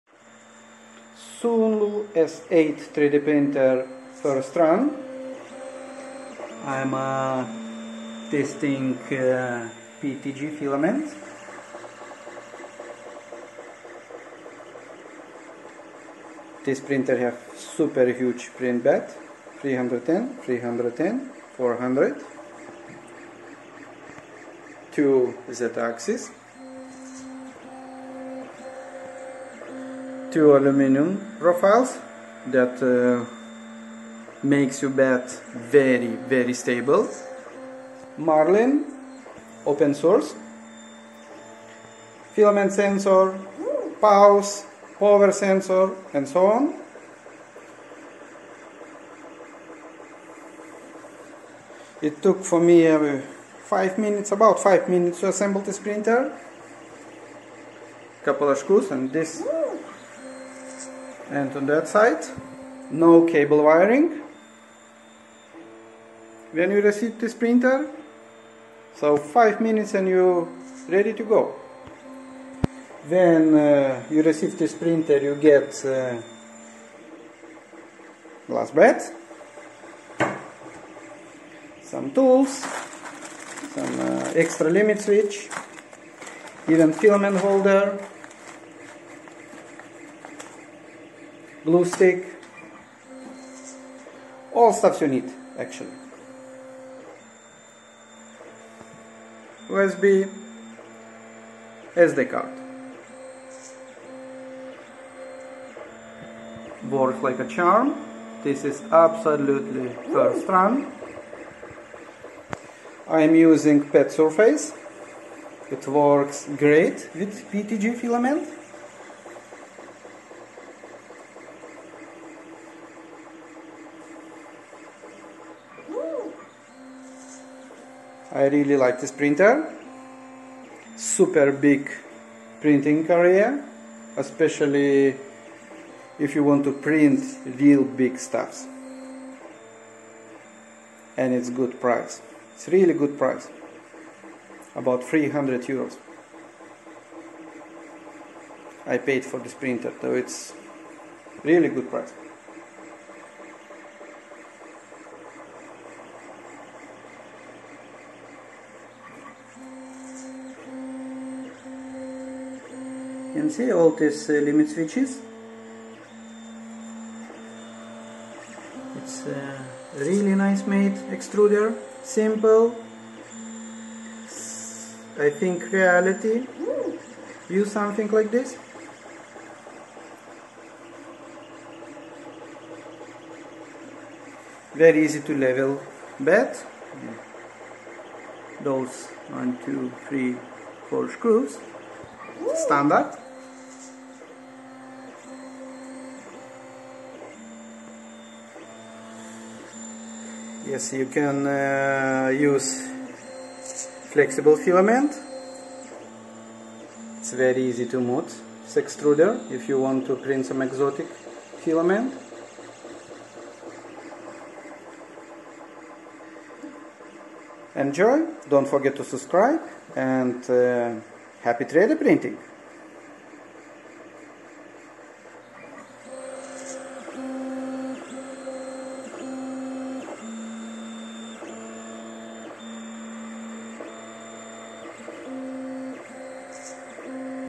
SUNLU S8, Large Format 3D Printer first run...
For what you get in a printer this one is hands down the best bang for the buck.
First off it has an enormousness print size
Second, it takes like no time to setup and calibration